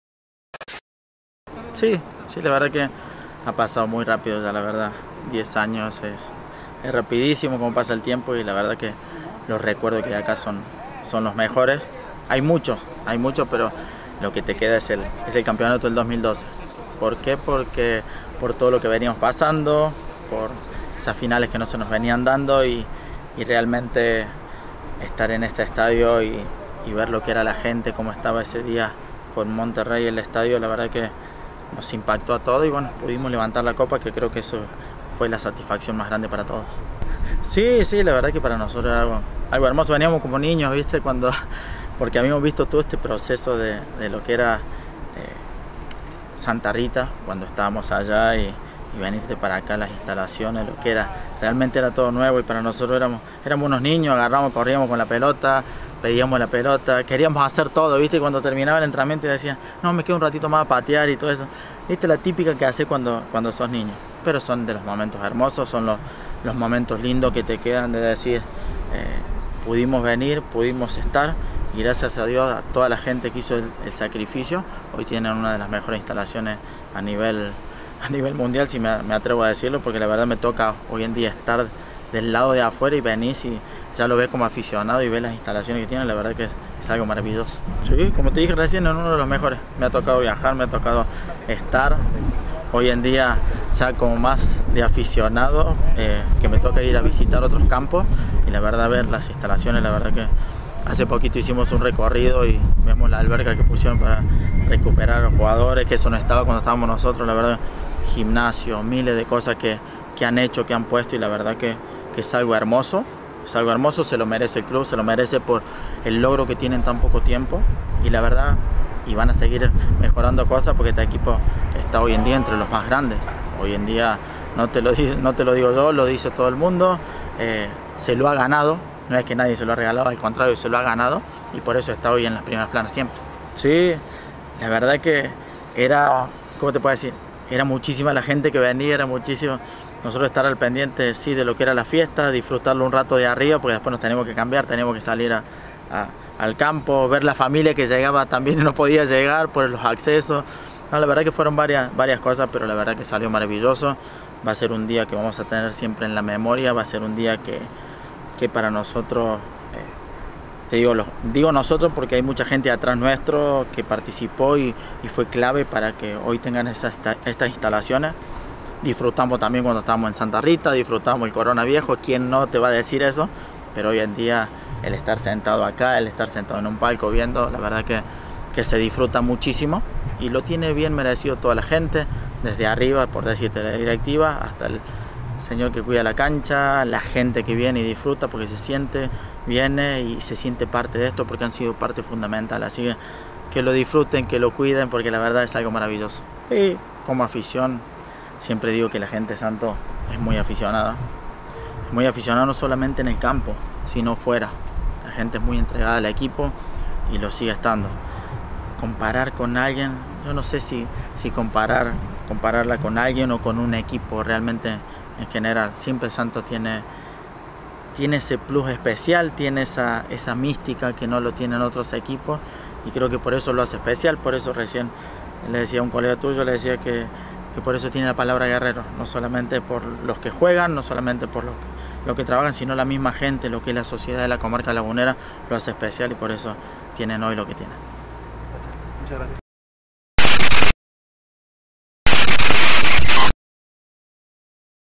Entrevista: Daniel Ludueña - 10º Aniversario TSM
El '10' hablando del '10'.
'Hachita' Ludueña nos platicó cuáles fueron los mejores momentos que ha vivido en esta primera década del Territorio Santos Modelo.